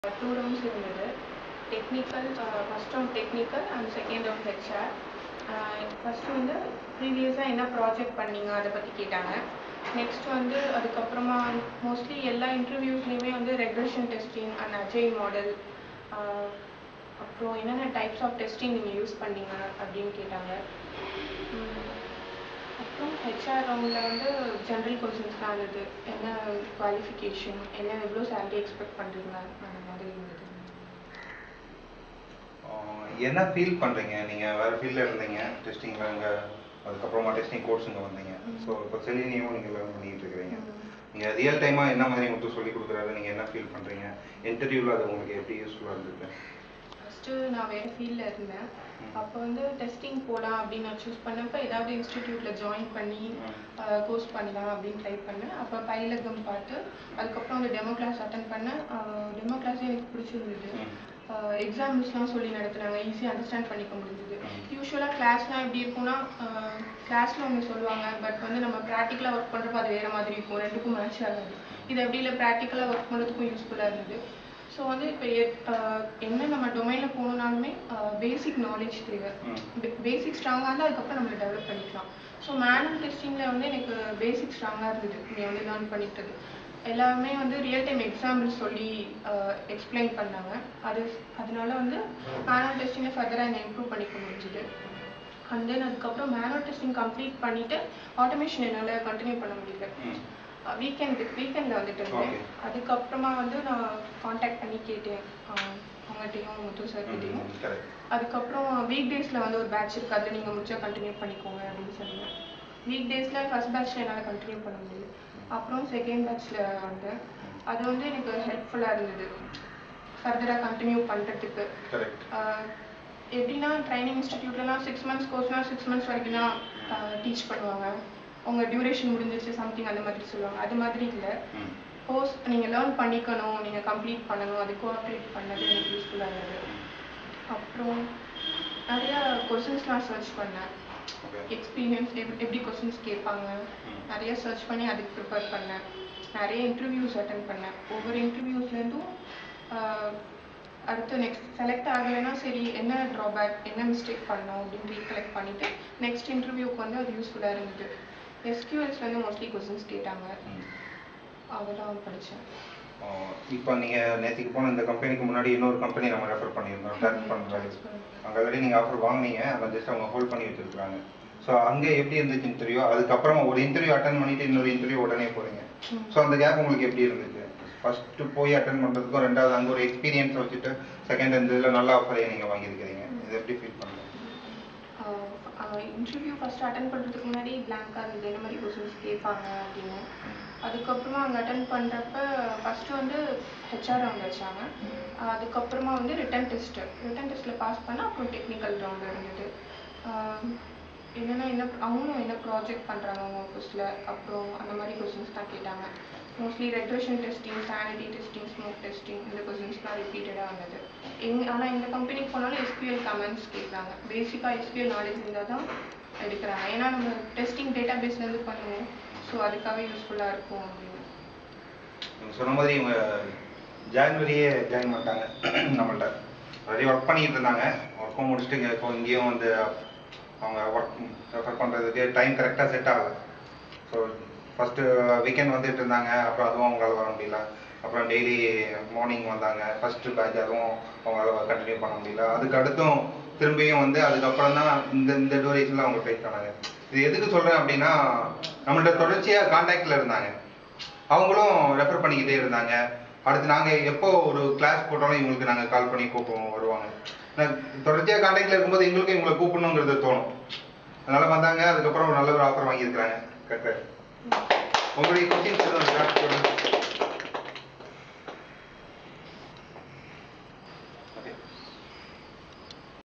Kiruthiga Devi (ICM Solutions) - Payilagam Placement Update - Software Testing Training in Chennai
PAYILAGAM PLACEMENT UPDATE:

Our Software Testing Trainee Mrs. Kiruthiga Devi got placed in ICM Solutions.

Our Software Testing Trainee Mrs.Kriuthiga Devi  got placed in ICM Solutions.  She shares her testing training experience here with.